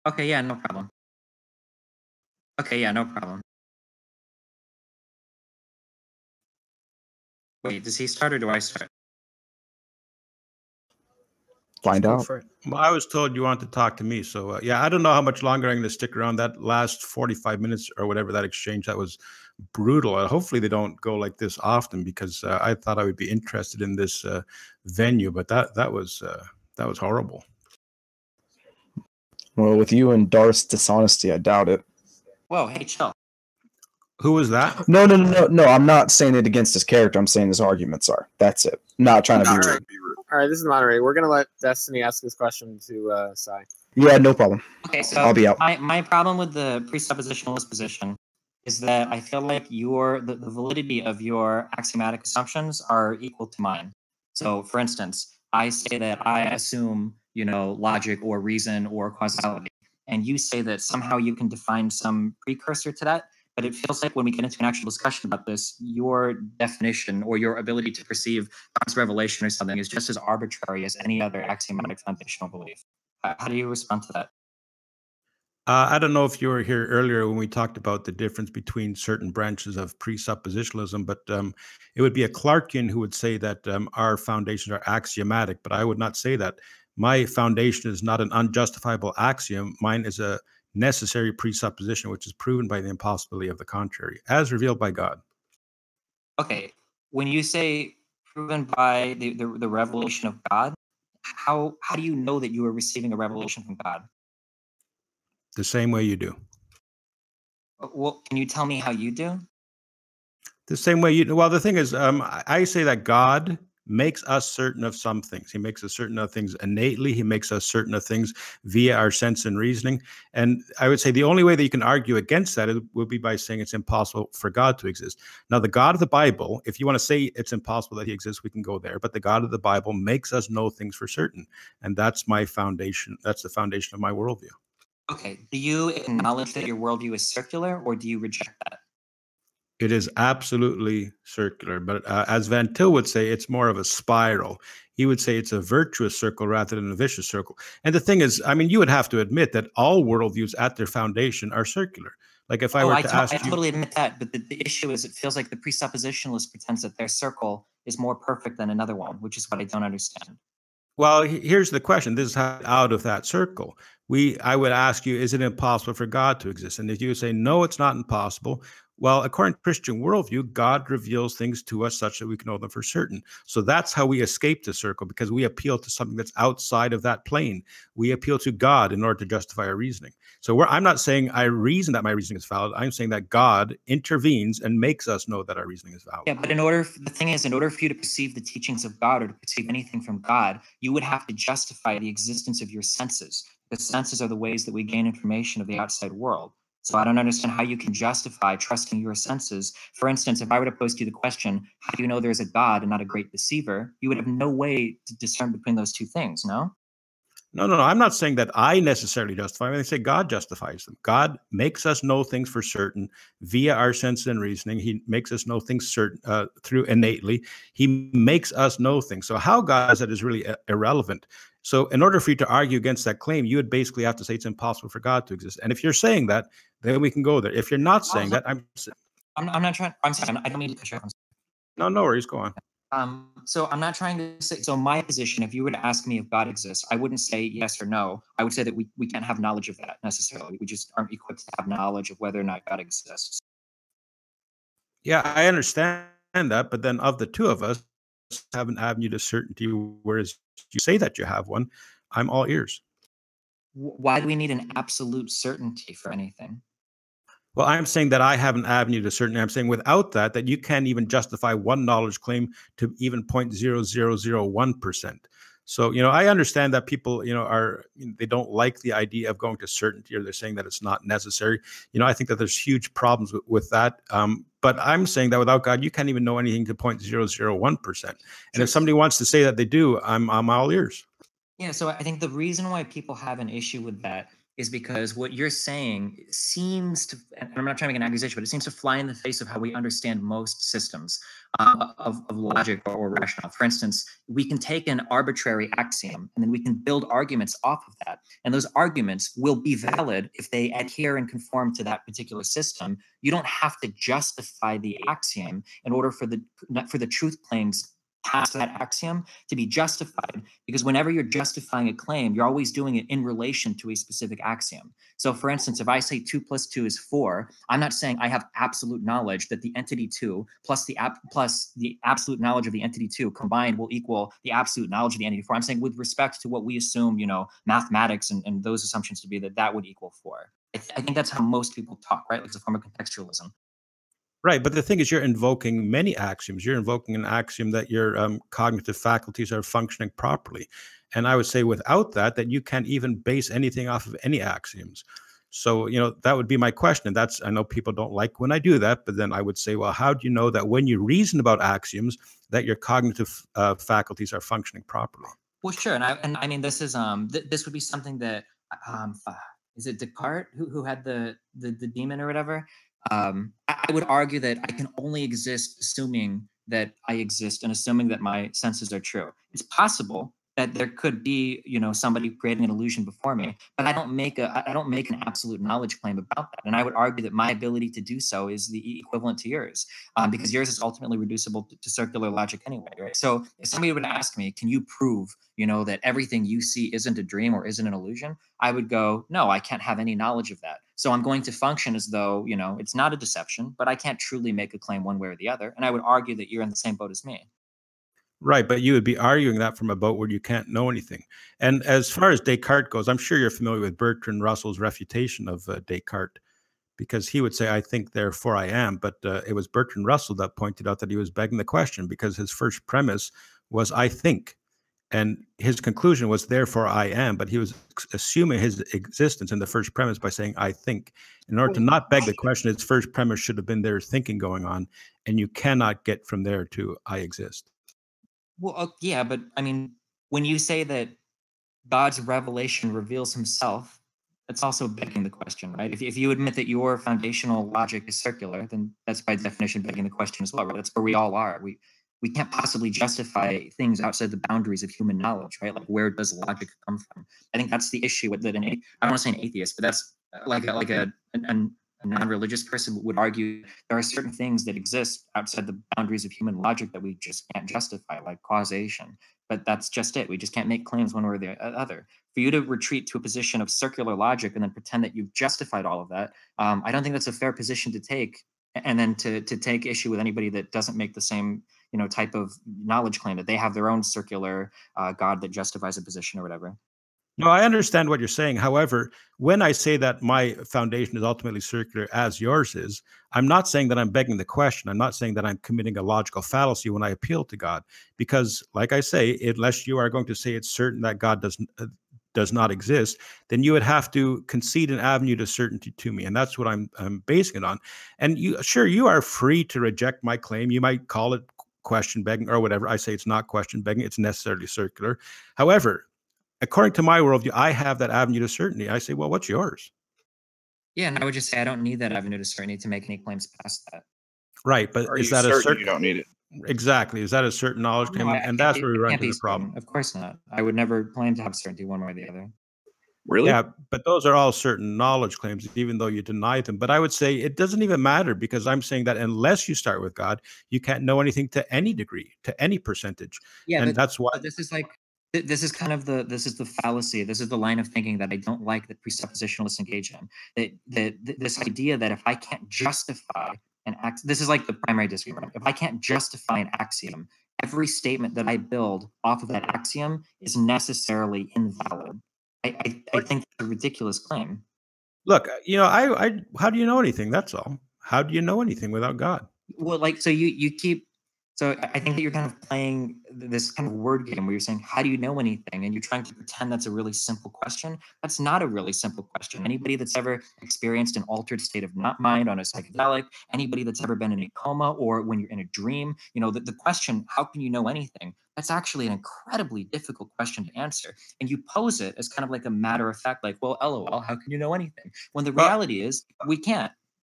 Destiny Gets Owned By Sye Ten Bruggencate
Debate between Sye Ten and Destiny on Discord in the Ask Yourself Discord server. Sye Ten is a Christian who uses presuppositional apologetics. Destiny is a Youtuber Debater.